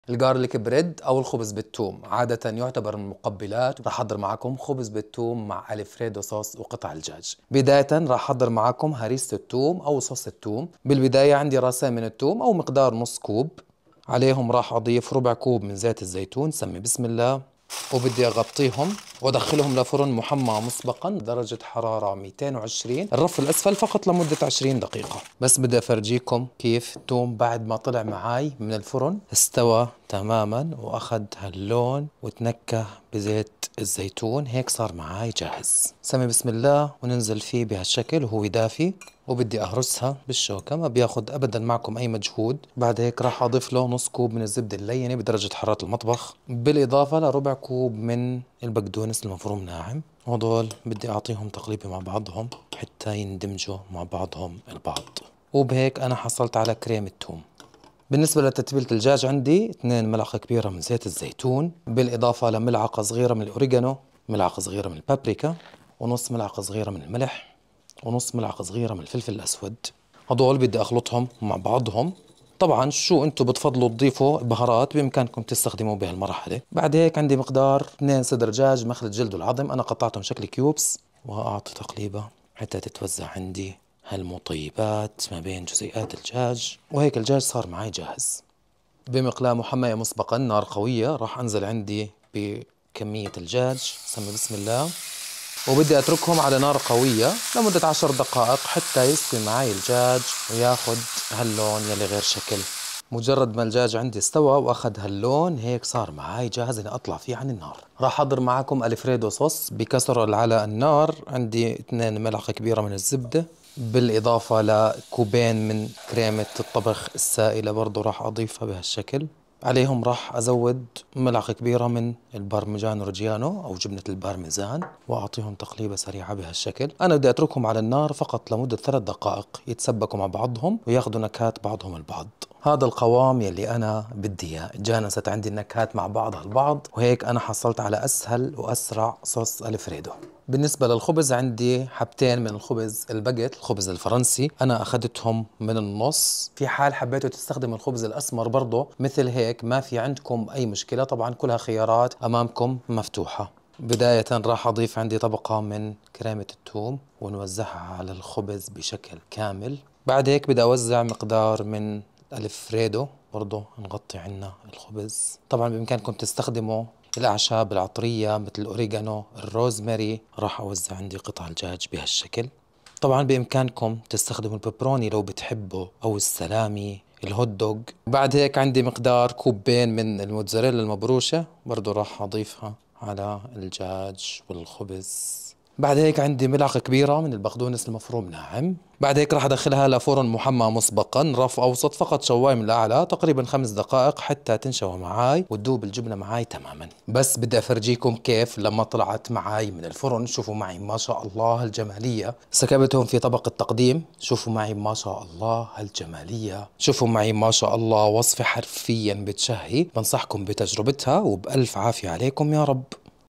خبز بالثوم بصوص الالفريدو الشهي سهل و سريع 🥰😋
مدة التحضير : 1 ساعة
الكمية تكفي : 4 أشخاص

2 حبة خبز باجيت
2 كوب جبن موتزريلا

تتبيلة الدجاج :
2 صدر دجاج كيوبس
2 م ك زيت زيتون
1م ص اوريغانو
1 م ص بابريكا
نصف م ص ملح
نصف م ص فلفل اسود

كريم التوم :
2 حبة توم مقشر
ربع كوب زيت زيتون
نصف كوب زبدة لينة
ربع كوب بقدونس

صوص الفريدو :
2 م ك زبدة
1 كوب كريمة طبخ سائل
1 م ك جبن بارميزان

طريقة التحضير :

1 - يوضع التوم في طبق الفرن ويضاف الزيت  ويحكم اغلاقه في القصدير يدخل فرن محمى مسبقآ درجة حرارة 220 لمدة 20د فور خروجه من الفرن يهرس التوم مع باقي المكونات

2 - يحضر تتبيلة الدجاج بخلط المكونات سويآ في وعاء ويضاف الدجاج ويقلب جيدآ

3 - في مقلاة محمية يشوح الدجاج مع التقليب نار قوية لمدة 10د ثم يوضع جانبآ

4 - في مقلاة محمية مسبقآ نار متوسطة توضع الزبدة والكريمة والجبن وتترك تتسبك لمدة 3د

5 - تضاف طبقة من كريم التوم على الخبز ثم طبقة من صوص الفريدو والجبن وقطع الدجاج يدخل فرن محمى مسبقآ رف اوسط شواي من الأعلى لمدة 5 د ويقدم دافئ .